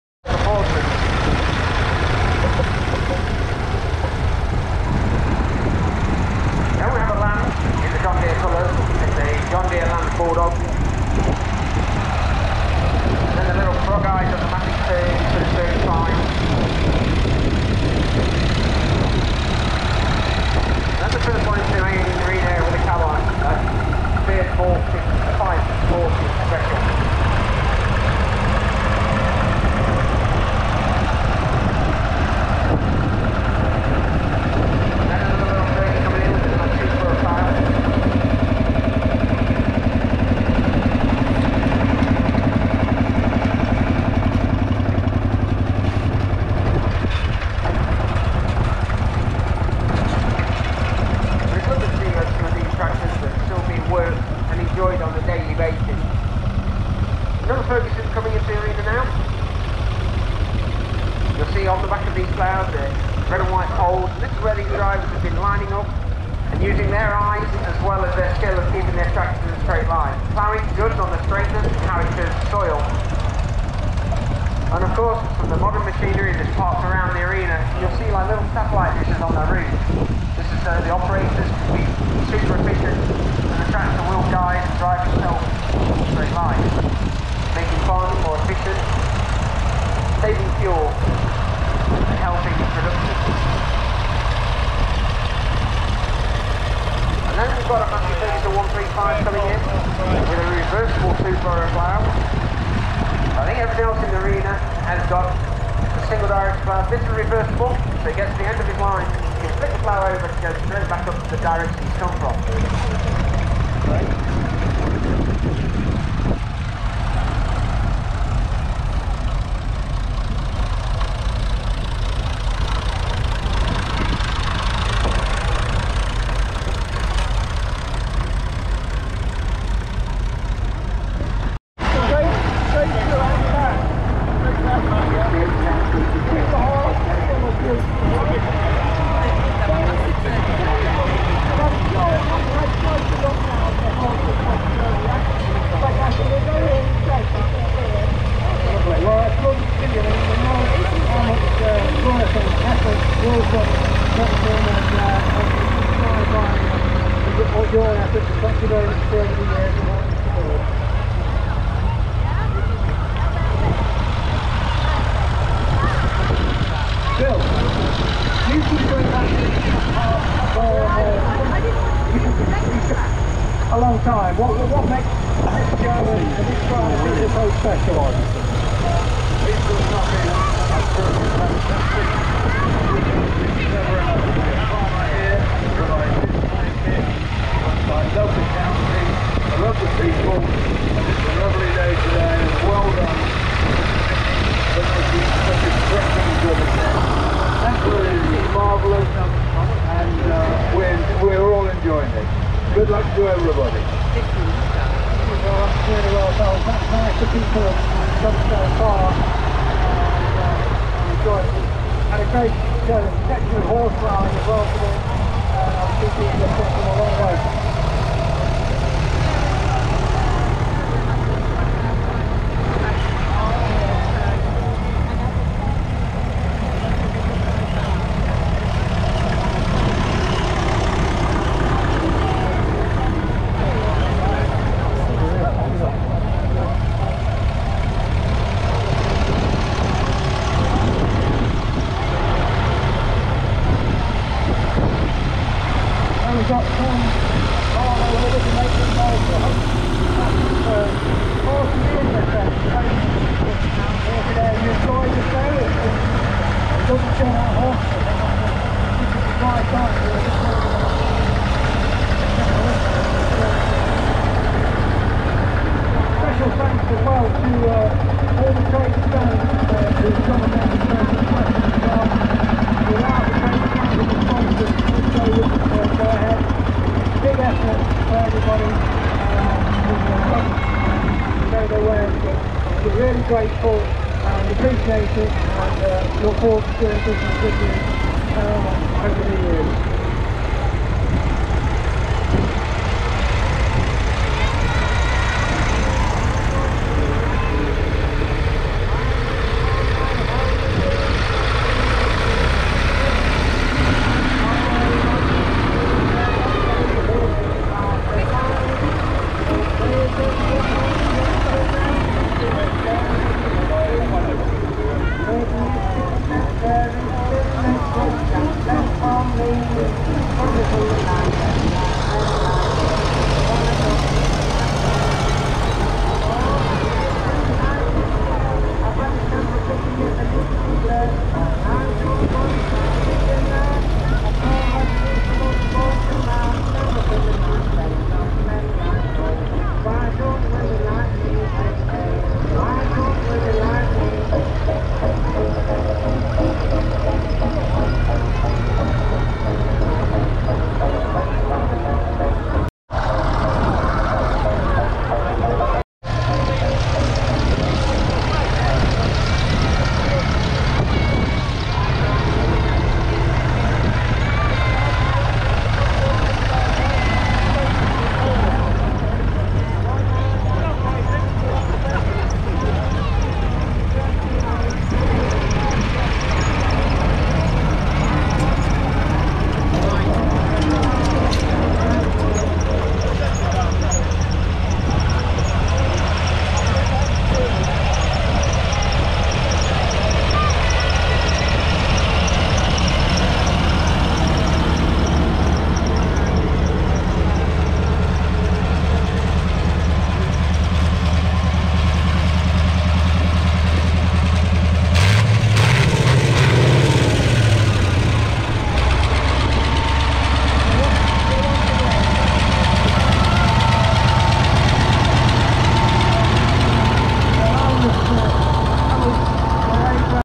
Tractors at the 2022 Flintham Show and Ploughing Match
100th Flintham and District Ploughing Match and Agricultural Show, Shelton, Nottinghamshire, England, United Kingdom - Thursday 22nd September 2022

Zetor, Ferguson, Lanz Bulldog, McCormick International Harvester, Fordson Super Dexta, Massey Ferguson, Fiat Tractors, Ford Tractors, David Brown, Roadless Burton 140